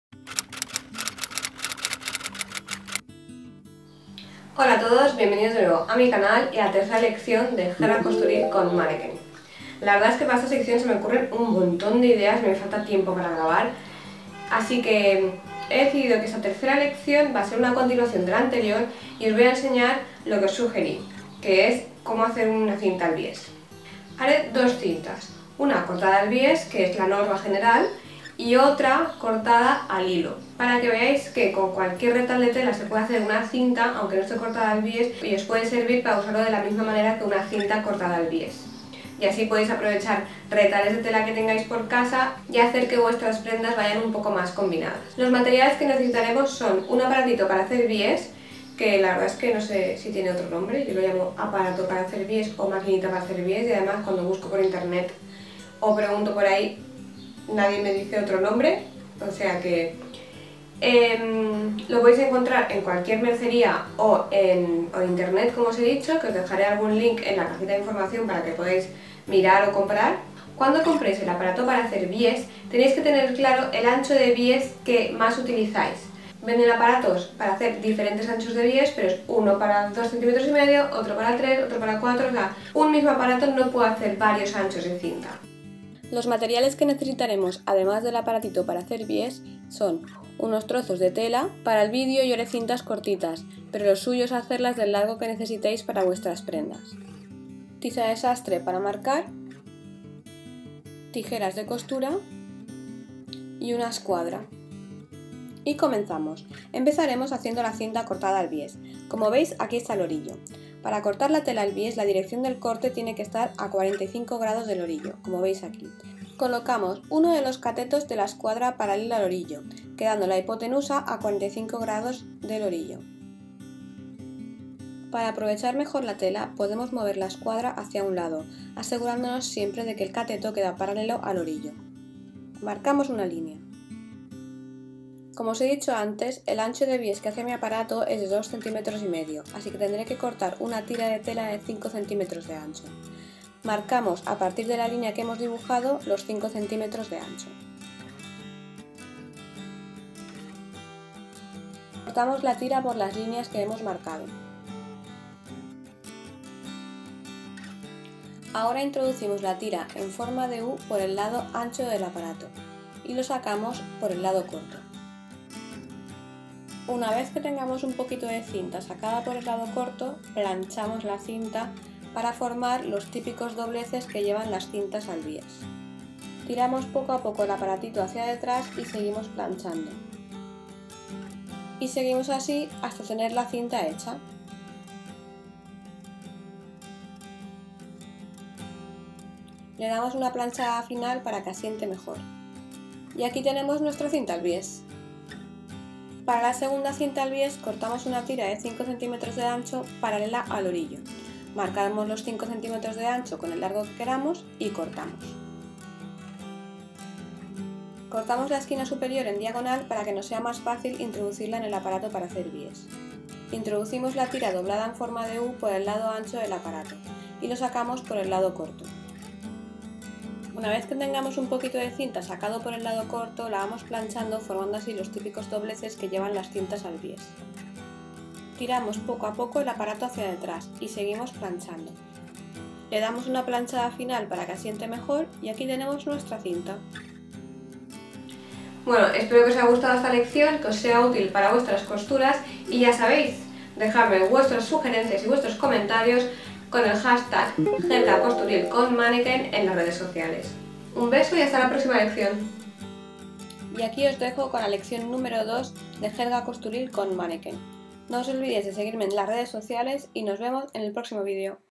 Lección 3 - Jerga Costuril - Como hacer una cinta al bies
En el vídeo de hoy os explico como hacer vuestra propia cinta al bies.
*** Pincha aquí***

* Yo soy el creador de este vídeo y todo el contenido del vídeo es creación mía.